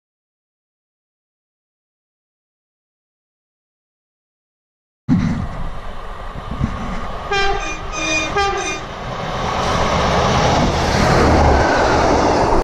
Network Rail 950 with .... Passing chester le street jan 2017
In this video we see the 950 head back south filmed sunday with a well known tune as he pass on the horn .
EXTRA VIDEO ONLY BY CLICK BELOW ***